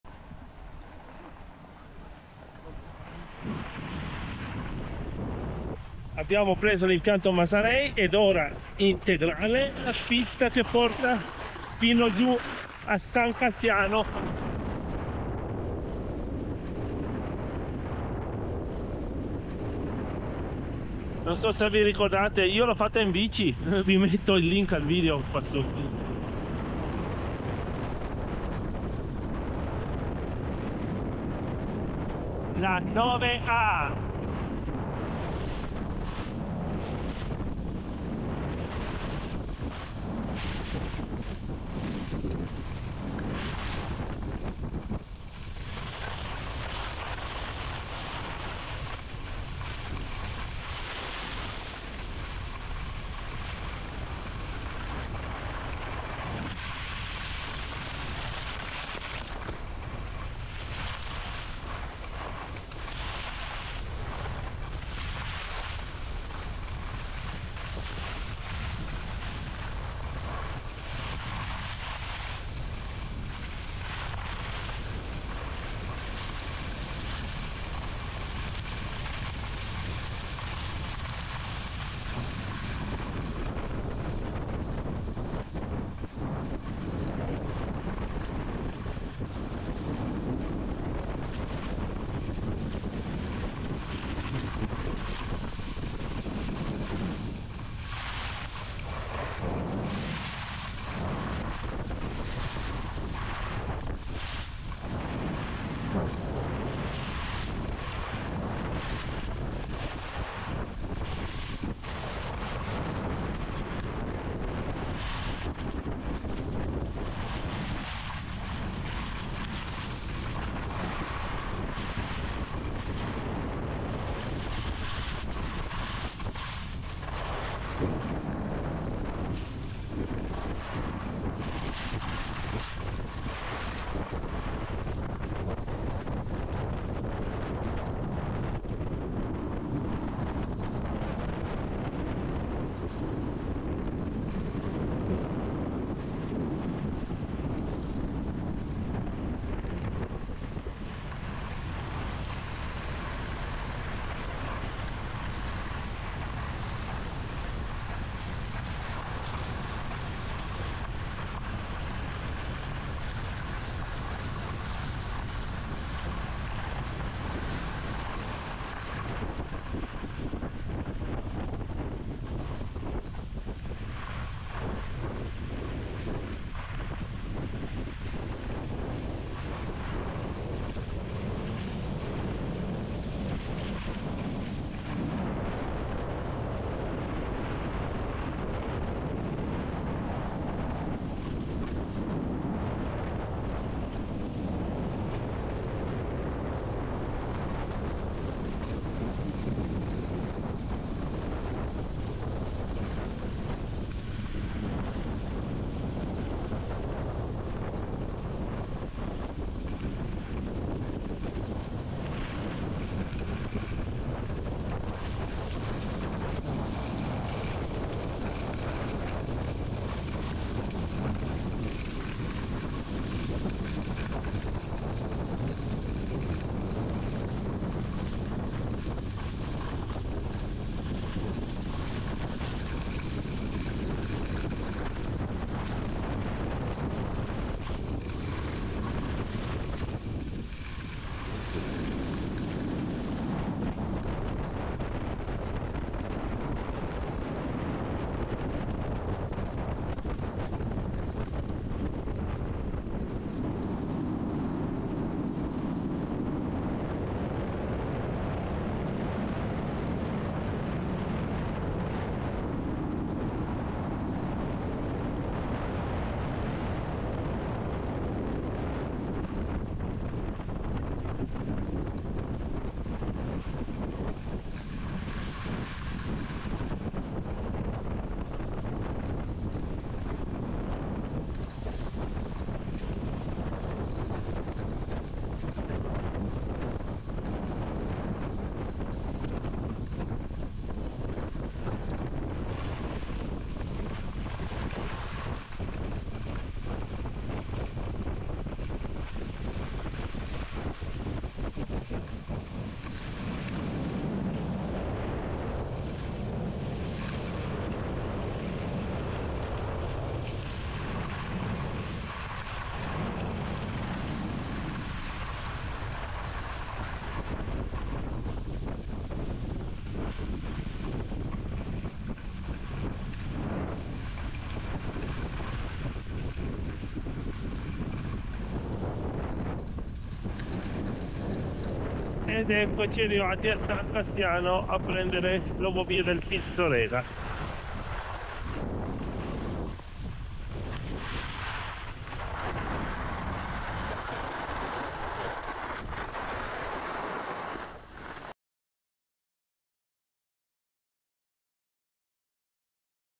Full 5km Descent on Alta Badia's Ski Run 9A: Pralongiá (2,157m) to San Cassiano
The Alta Badia Ski Run 9A is categorized as a blue run. It is one of the longest ski runs in the area and it is really easy. On top of that, you can always see the Sas dla Crusc and Conturines massifs right in front of you, so the entire run feels like being in a theater watching an amazing nature documentary. Don't believe it?

PS: By the way, did you know that this slope is 'so easy' that I've had no problem cycling down it with a bike on snow? Don't believe this either? Well, check out this video then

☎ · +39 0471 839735
📥 · info [at] ustariaposta.it